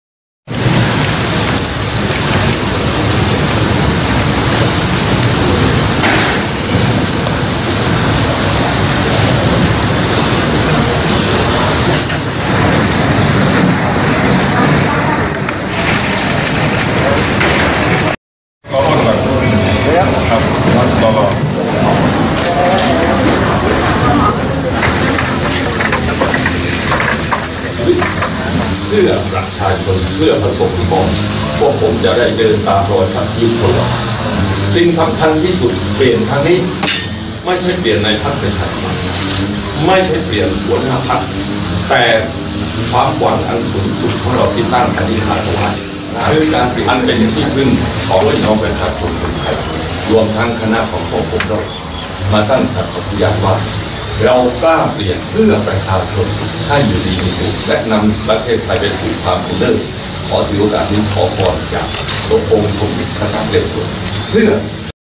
“หมอวรงค์-คณะกล้าเปลี่ยน”ลั่นฆ้องหน้าพระองค์ดำ เอาฤกษ์เปิดตัวชิงเก้าอี้ หน.ปชป.
27/09/2018 “หมอวรงค์-คณะกล้าเปลี่ยน”ลั่นฆ้องหน้าพระองค์ดำ เอาฤกษ์เปิดตัวชิงเก้าอี้ หน.ปชป.
พิษณูโลก – “หมอวรงค์ – คณะกล้าเปลี่ยน” กราบสักการะหลวงพ่อพระพุทธชินราช-องค์สมเด็จพระนเรศวรมหาราช พร้อมลั่นฆ้องเอาฤกษ์เอาชัยหน้า “พระองค์ดำ” ก่อนเปิดตัวชิงตำแหน่งหัวหน้าพรรคประชาธิปัตย์เย็นนี้ที่ ม.นเรศวร